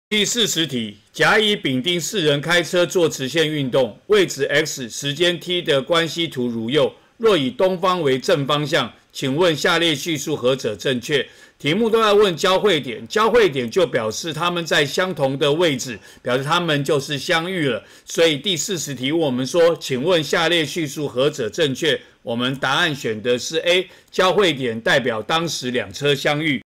109學年上9年級理化一段第40題
以東方為正，給四人做直線運動x-t圖。問交會點的意義。